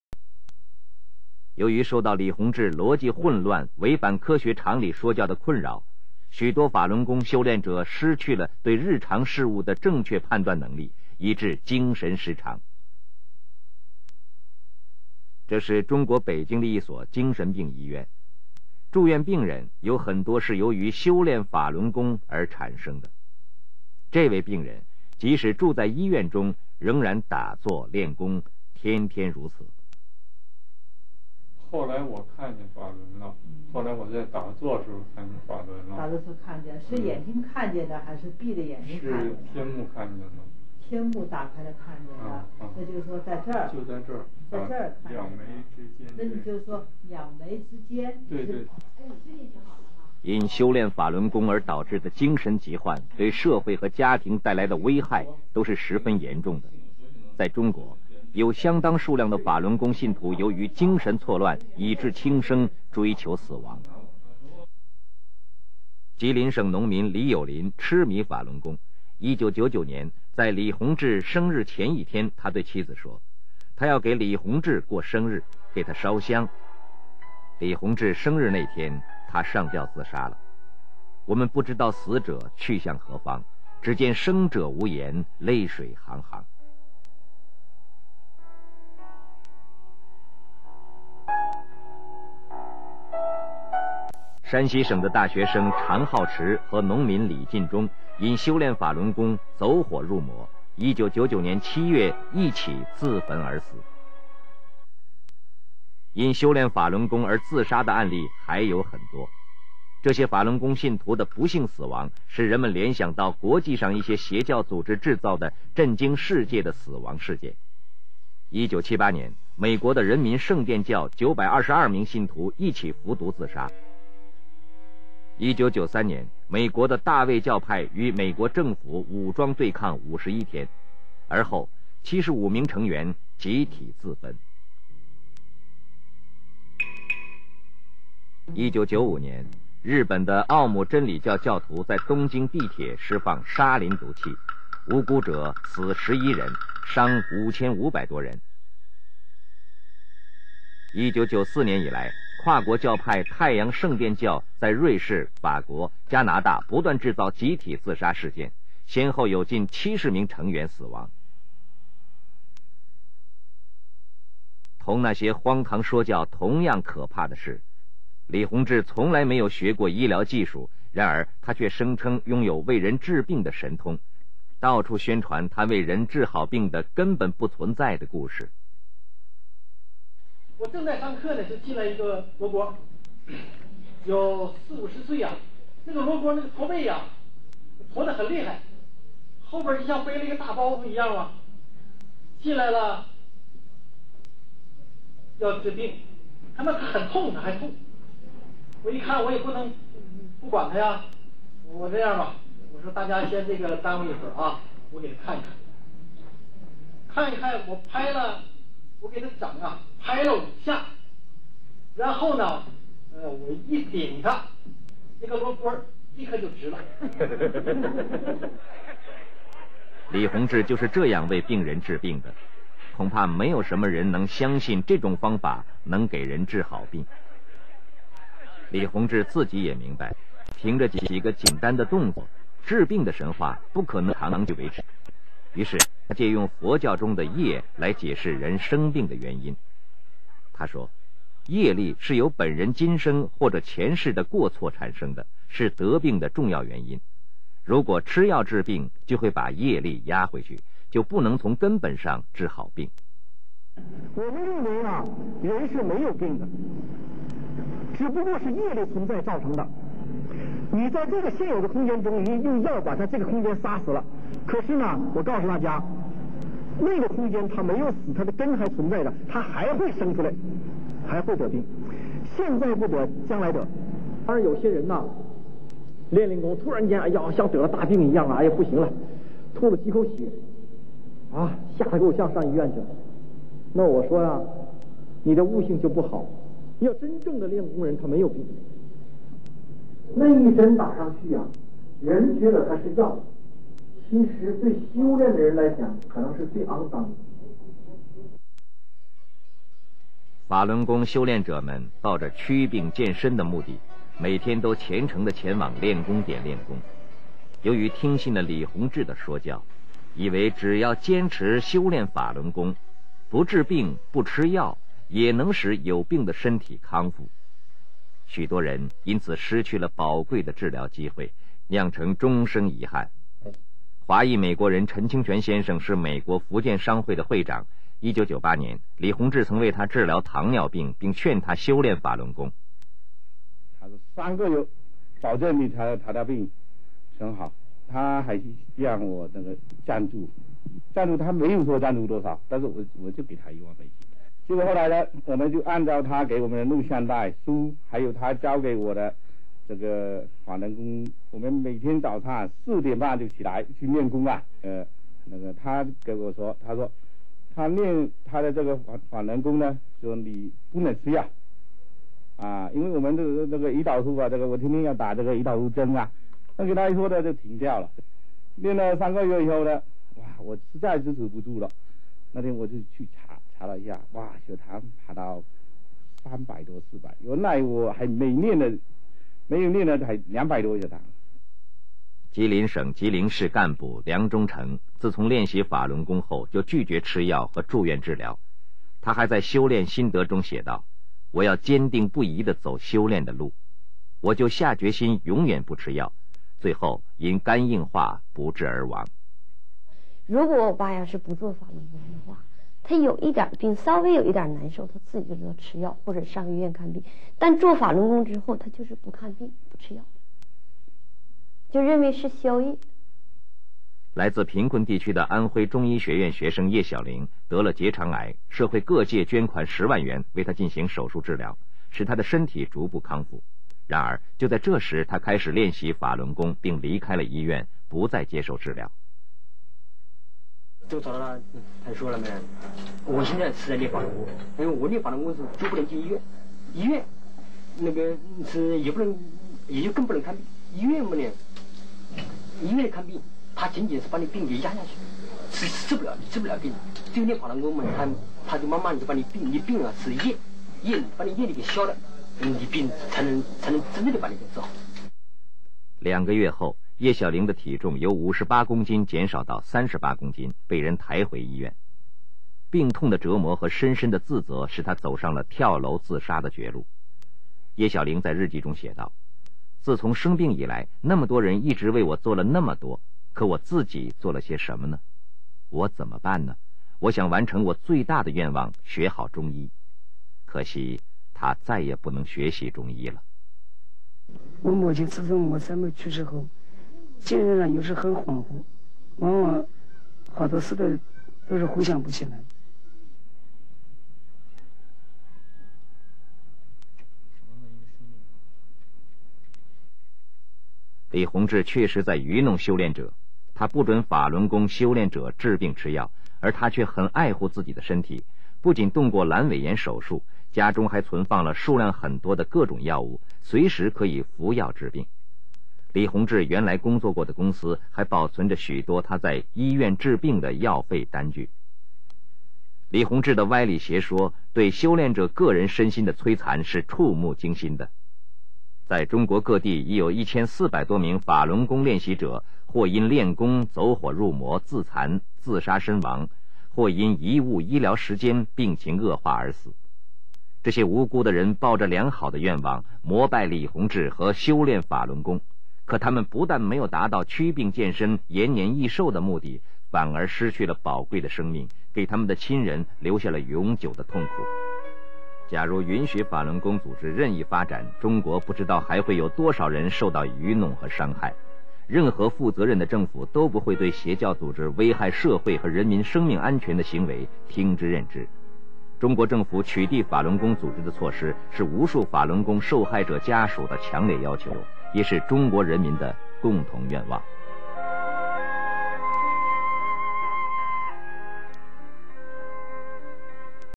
The Cult Falun Gong 02
In 1992, a cultic organization came into being and spread quickly across China. It upheld the banner of Truthfulness, Compassion and Tolerance, and attracted for a time millions of followers in the name of keeping fit. This cult was called Falun Gong, and its founder named Li Hongzhi. .